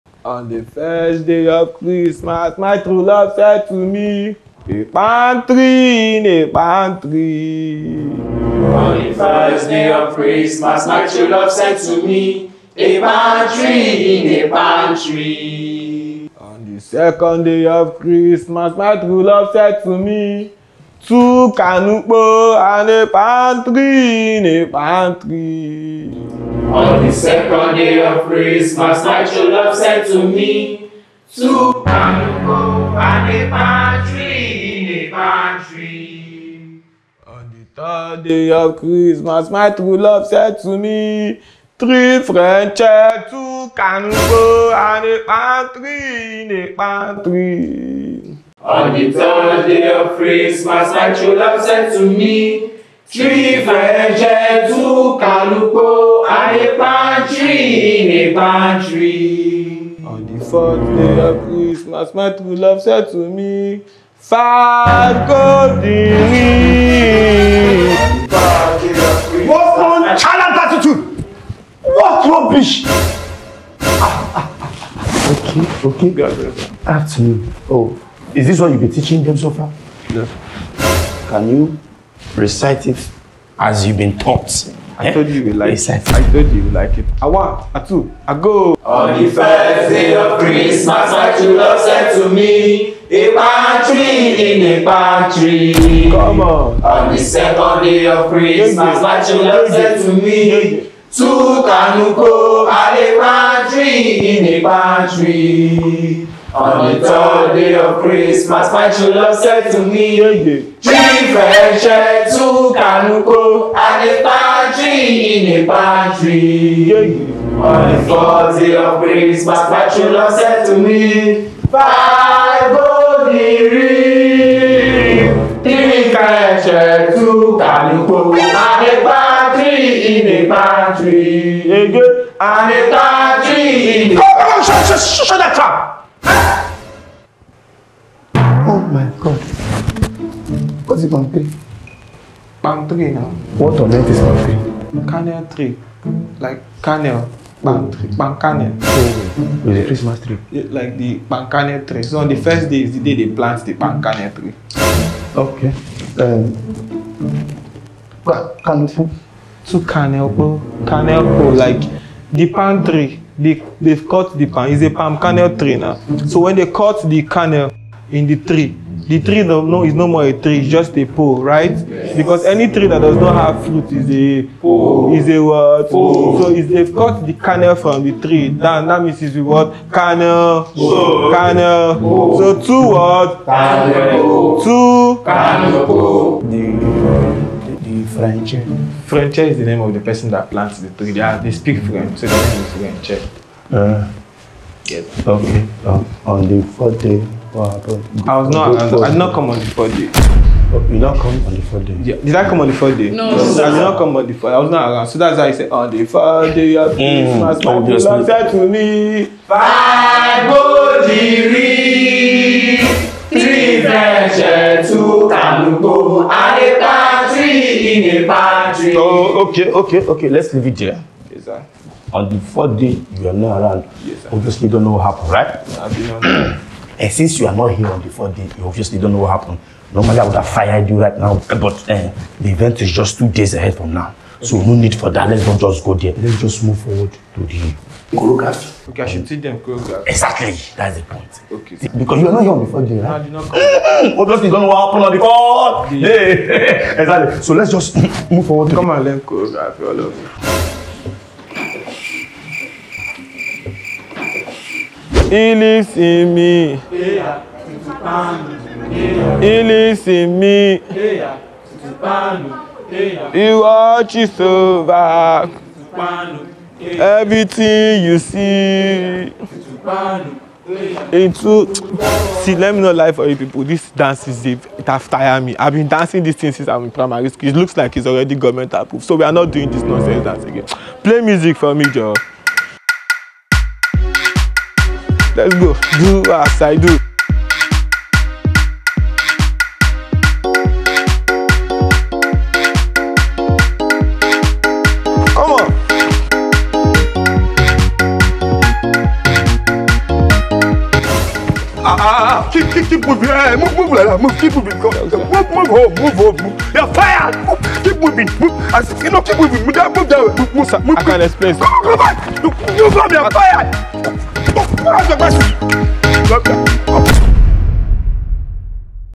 Brain Jotter - THE CHRISTMAS CHOREOGRAPHY
Brainjotter Teaches his students on Christmas choreography